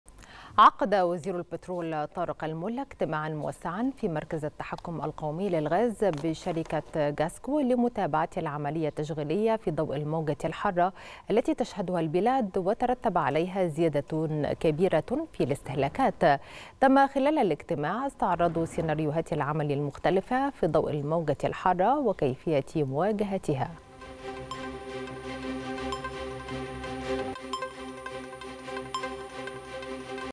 وزارة البترول تستعرض سيناريوهات العمل المختلفة في ضوء الموجة الحارة
نشرة الثالثة | وزارة البترول تستعرض سيناريوهات العمل المختلفة في ضوء الموجة الحارة